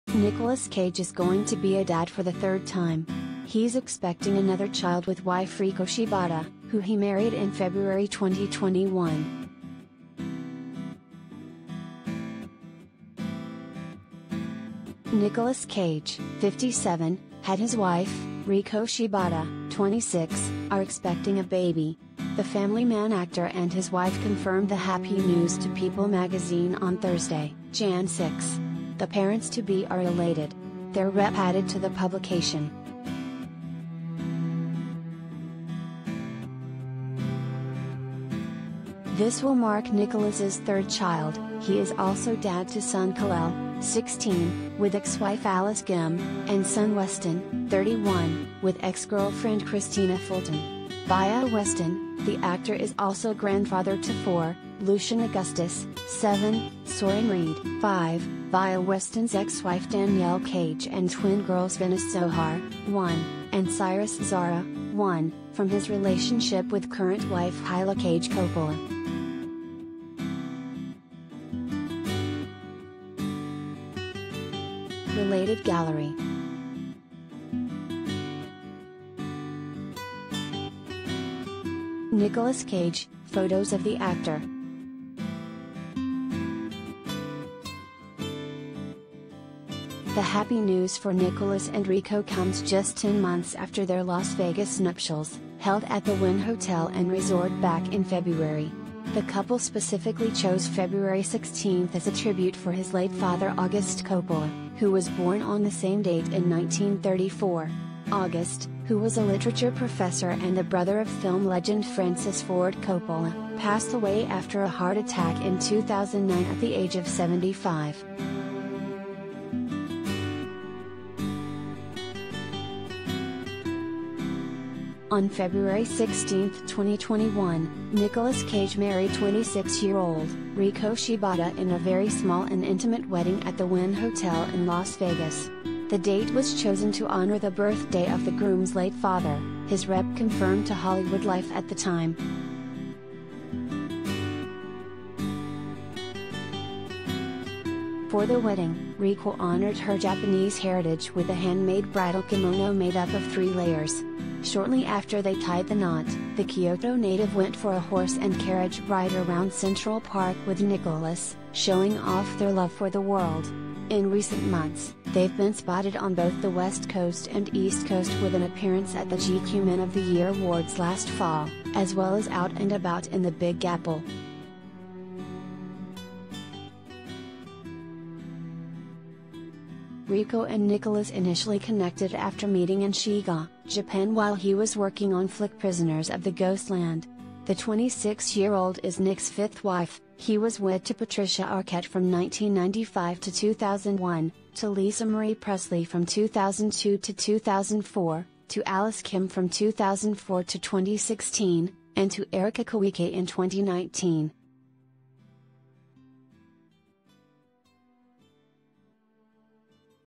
Nicolas Cage’s Wife Riko Shibata Pregnant: Actor Is Expecting Baby No. 3
Nicolas Cage is going to be a dad for the third time! He’s expecting another child with wife Riko Shibata, who he married in February 2021.
Nicolas Cage, 57, had his wife, Riko Shibata, 26, are expecting a baby! The Family Man actor and his wife confirmed the happy news to PEOPLE magazine on Thursday, Jan 6. “The parents-to-be are elated!” their rep added to the publication.
This will mark Nicolas’s third child: he is also dad to son Kal-El, 16, with ex-wife Alice Kim, and son Weston, 31, wi